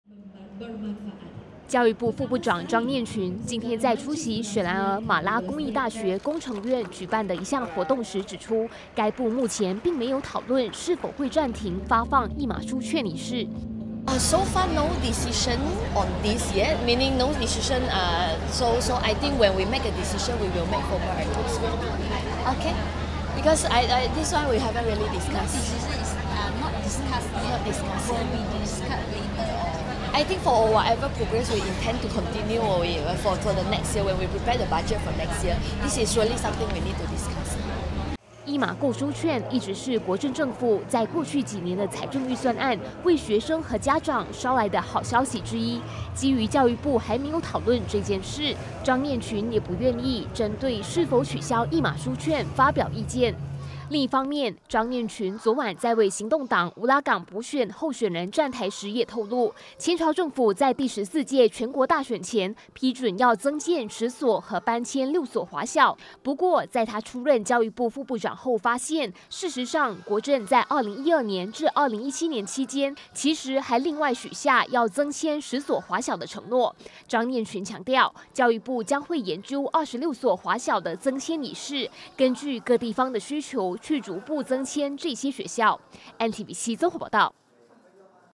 一马书券是否取消？ 张念群：暂未讨论
随着首相敦马哈迪日前宣布，政府探讨逐步取消发放一马人民援助金，那政府的另一项援助计划，也就是发放给学生的一马书券，是否也会被废除？引起高度关注。教育部副部长张念群今天回应，目前教育部还没有t讨论是否要废除一马书券这项惠民举措，不过任何有关明年会继续的项目，该部将会开会讨论。

教育部副部长 张念群：“目前没有就这事做任何决定(（取消一马书券），当我们有决定我们会宣布，不过这个课题我们暂时没有讨论。我相信任何我们决定要继续的计划，我们会在准备明年的财政预算时检讨。”